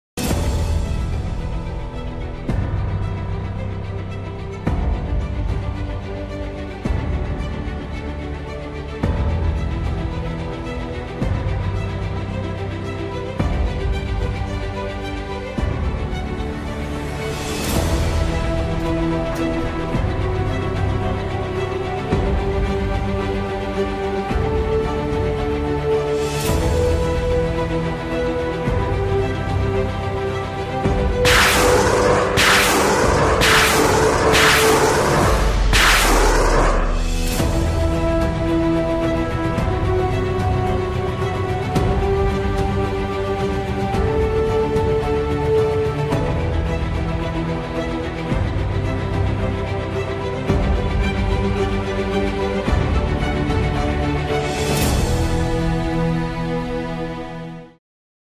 Tek sette 12 roket ateşleyen VURAN hazır
Yerli ve milli teknolojilerle üretilen VURAN aracına entegre edilen 107 mm'lik çok namlulu roketatar sistemi test atışında hedefi başarıyla vurdu.